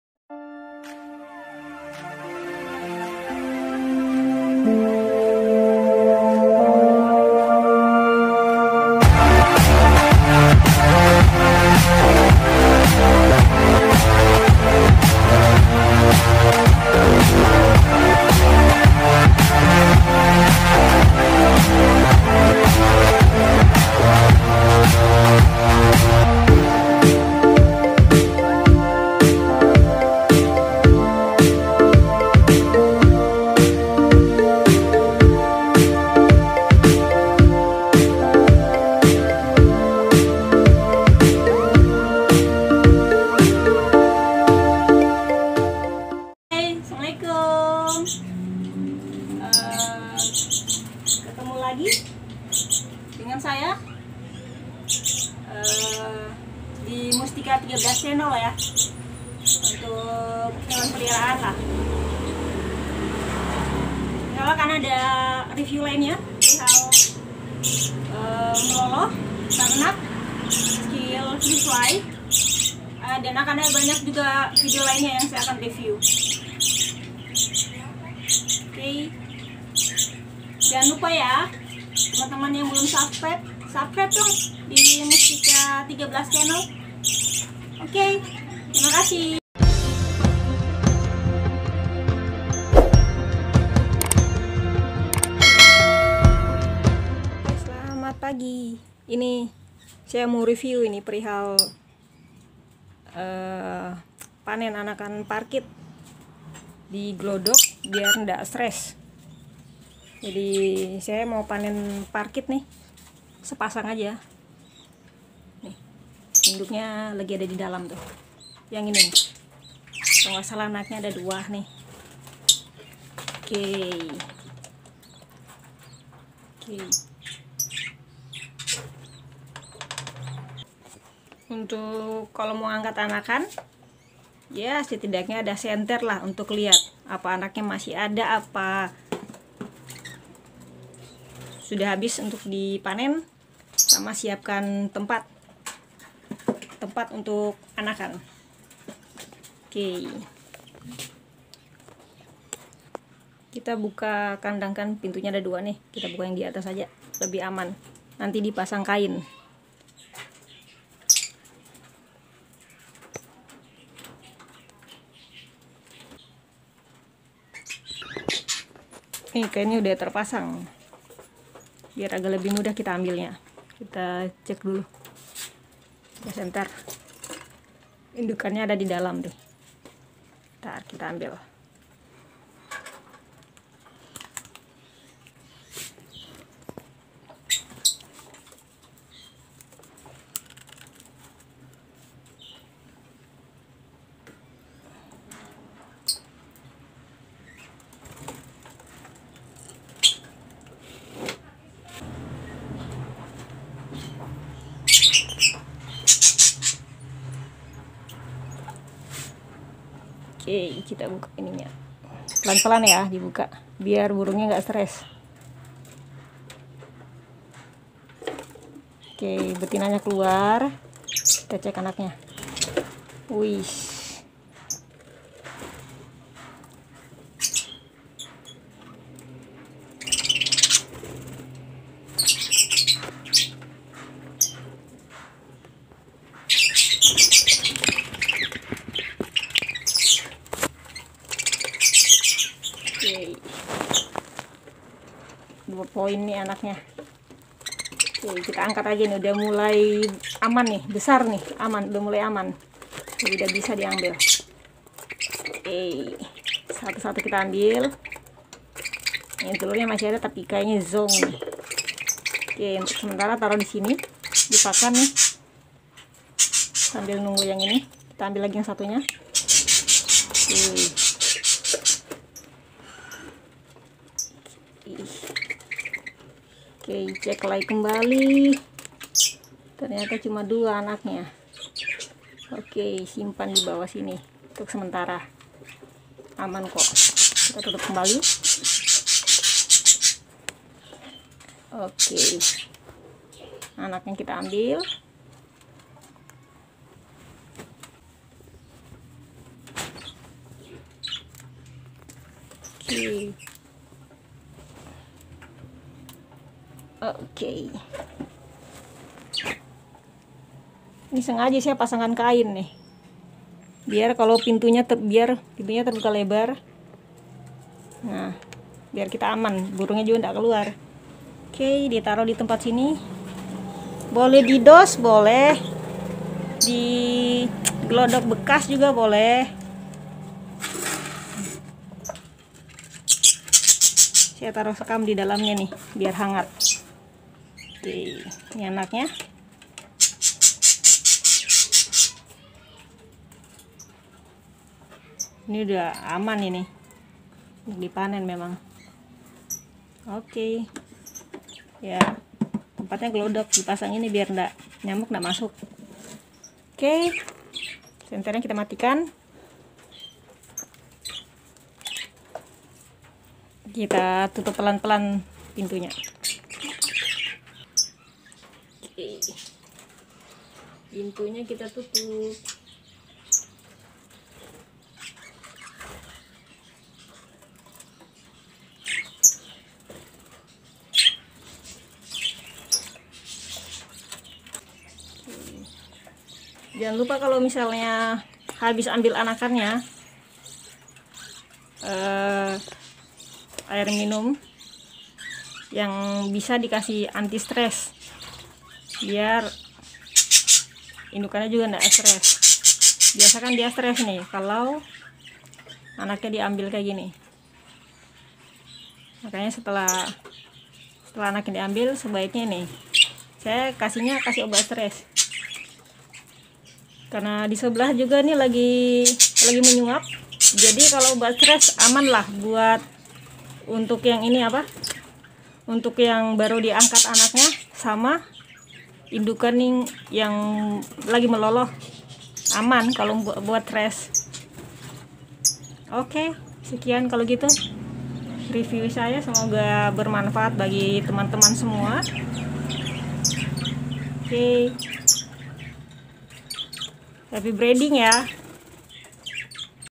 Proses Pengangkatan anakan burung parkit
Cara n solusi panen anakan ternak (Parkit) biar aman
Agar indukan tidak mudah stres
burung parkit memiliki paruh seperti tampak pada gambar bentuk paruh seperti itu berfungsi untuk
burung parkit yang pada mulanya sepasang sekarang menjadi banyak karena